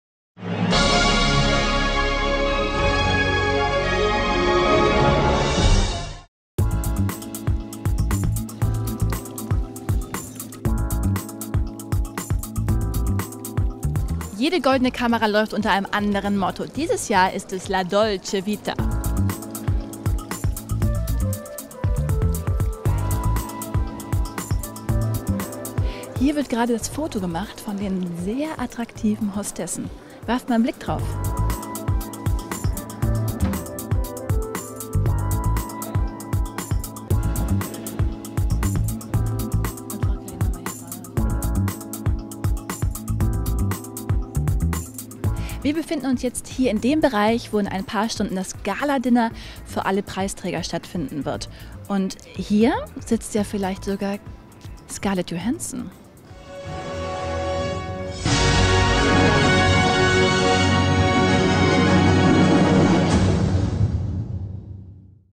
Hinter den Kulissen - Goldene Kamera 2012
Moderatorin Sophie von Hoyningen-Huene hinter den Kulissen der Galaveranstaltung.